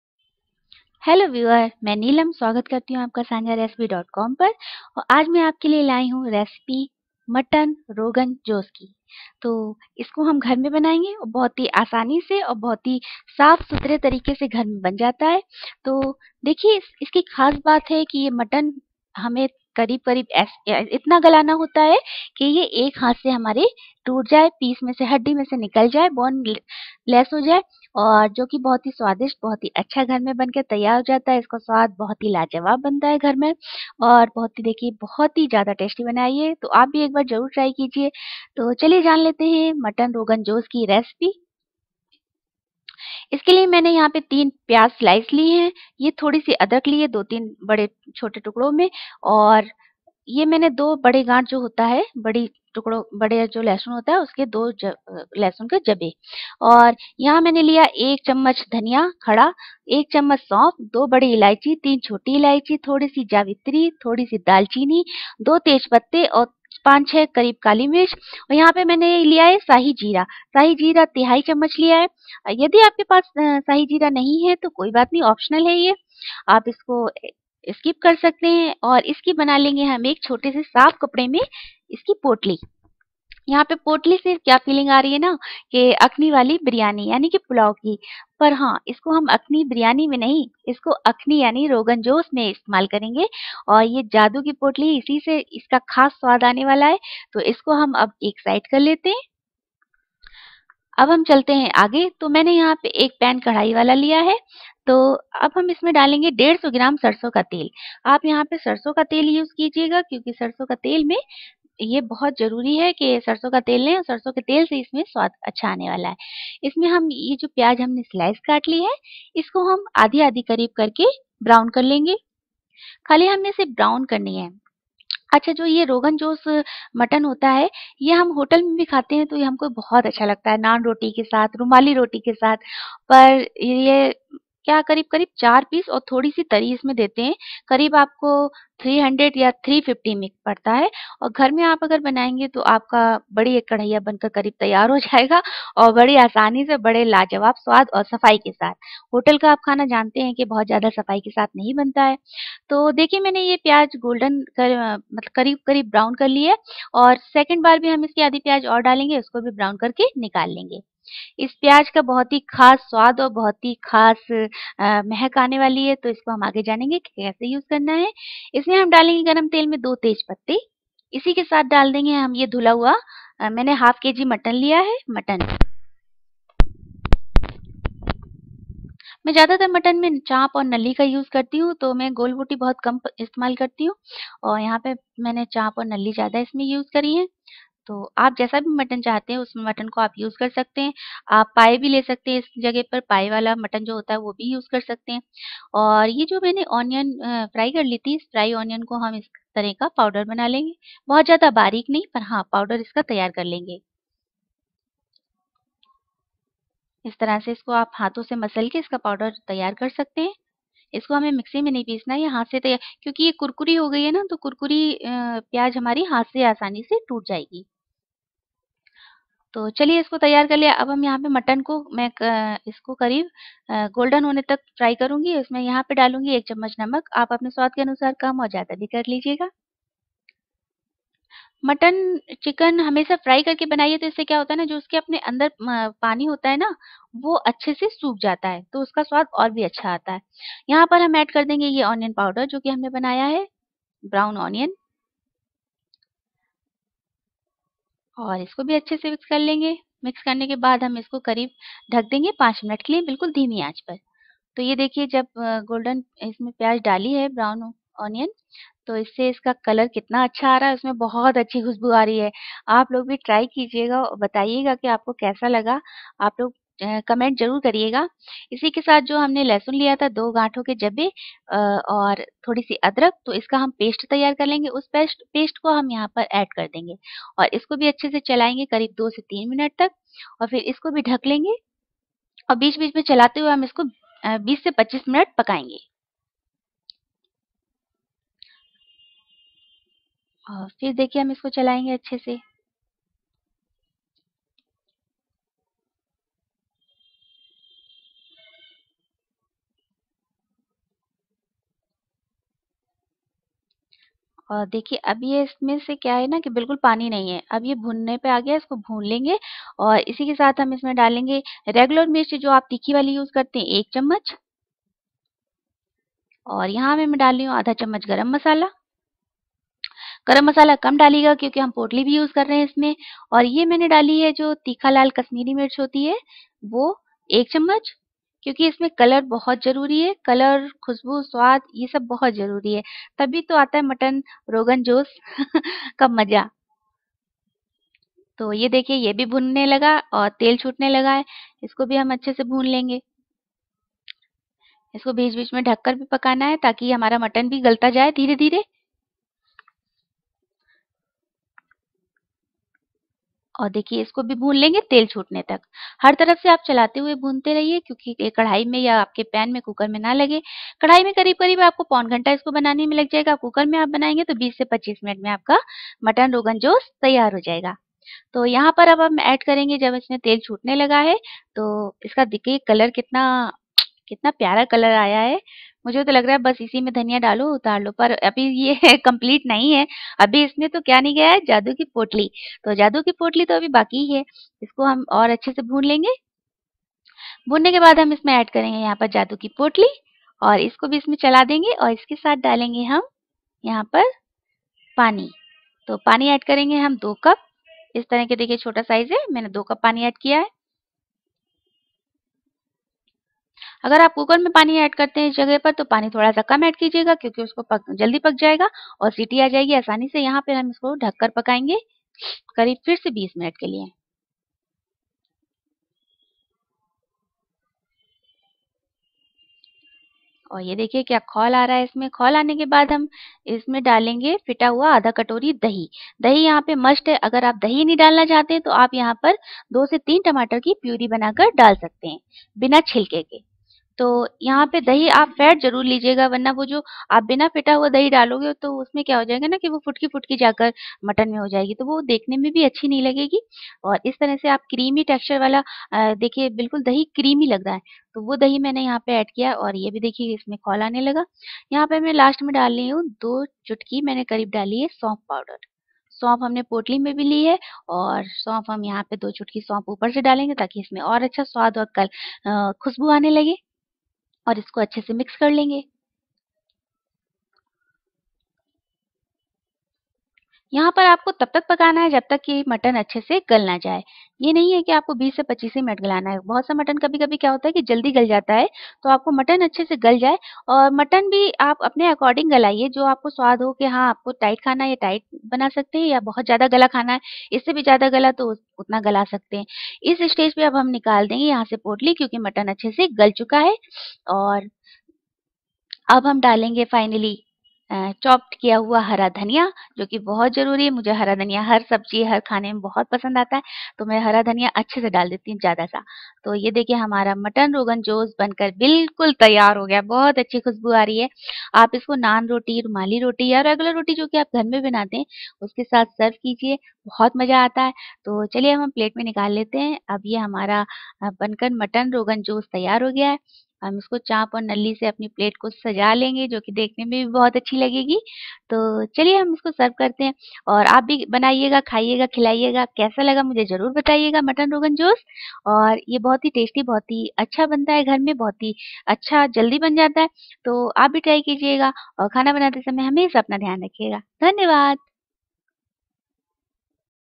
Recipe Mutton Rogan Josh || घर पर बनाये
Is tarike se ham badi hi asaan tarike se ghar par hi tasty Reicipe mutton rogan josh bana sakate hein.

Ingredients
3 pyaj (sliced )
3 bade tujade adrak
2 bade lahsun
1 chamah khada dhaniya
1 chamach saunf
2 badi ilaihi
3 chhoti ilaihi
Thodi si- Dalchini , Javetri
2 tej patte
5-6 kali mirch
1/3 chamach sahi jeera (optional)
150 gram sarson ka tel
1/2 kg mutton
1 chamach namak
1 chamach lal mirch powder regular wala
1/2 chamach garam masala
1/2 katori phitta dahi
2 chutaki sauf powder